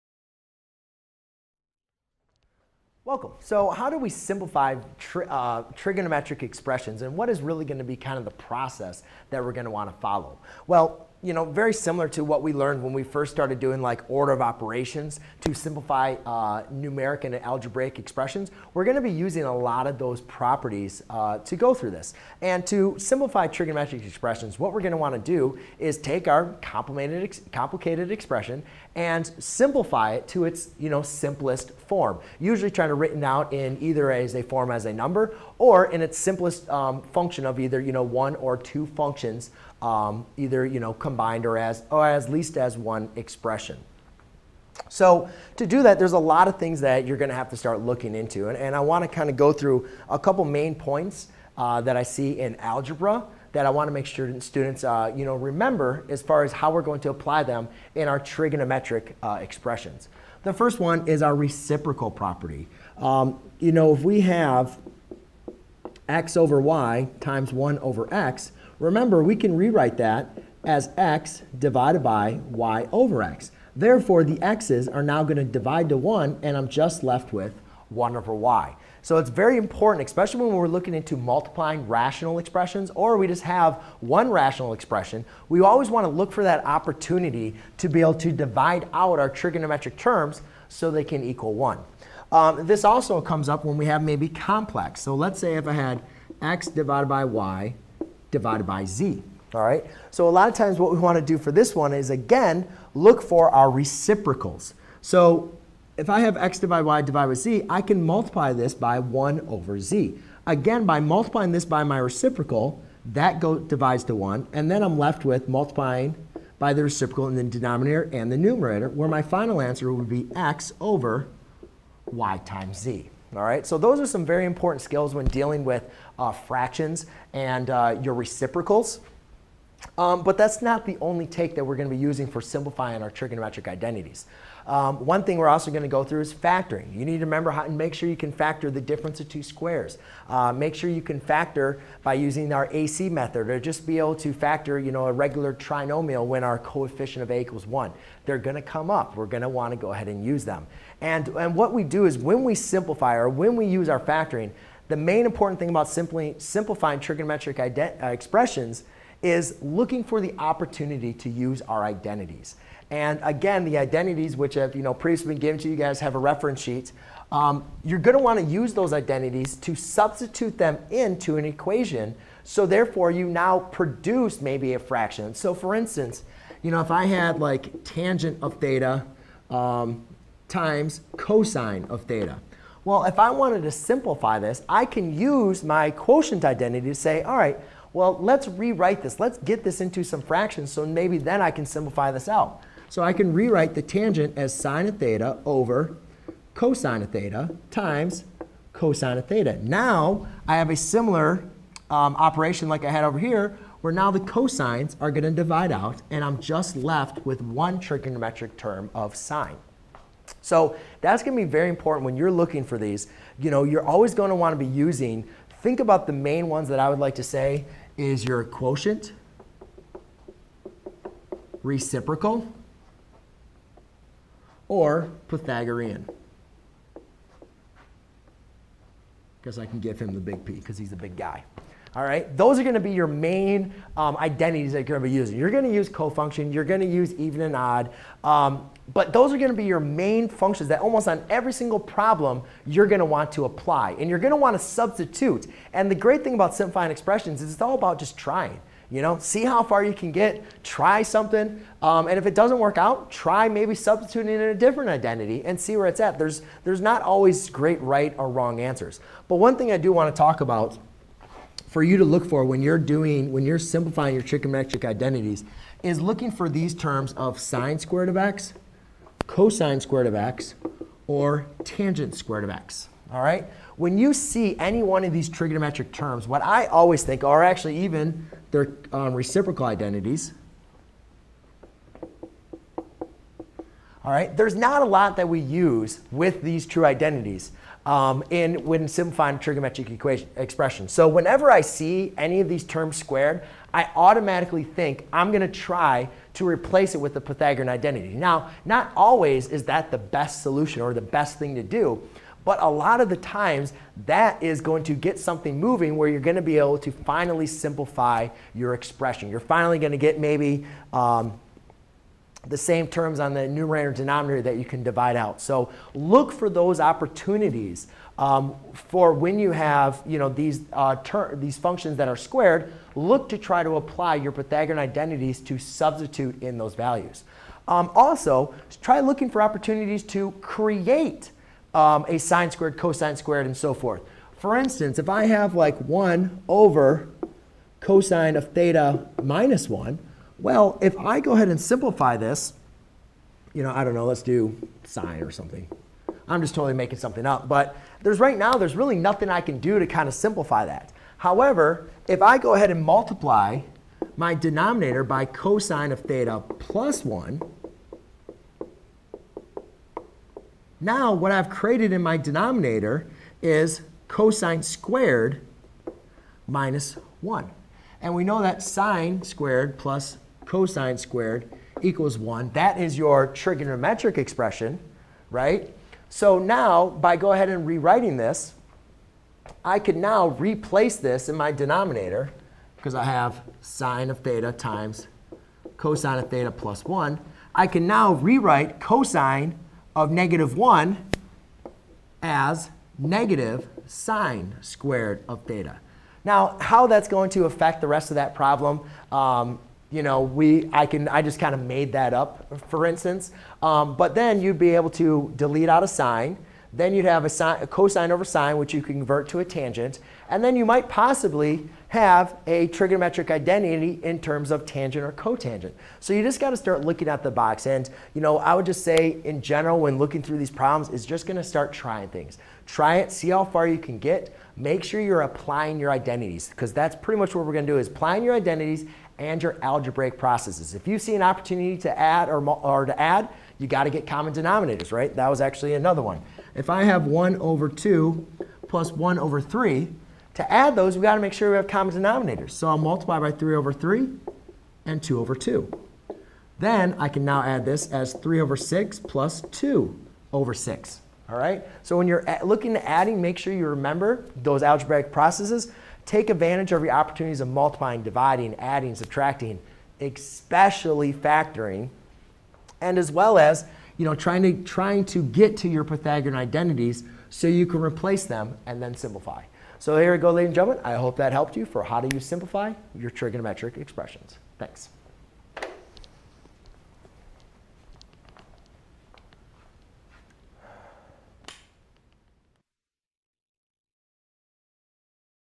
How to Simplify Trigonometric Expressions
👉 Learn all about the different trigonometric identities and how they can be used to evaluate, verify, simplify and solve trigonometric equations.  The identities discussed in this playlist will involve the quotient, reciprocal, half-angle, double angle, Pythagorean, sum, and difference. Identities help us rewrite trigonometric expressions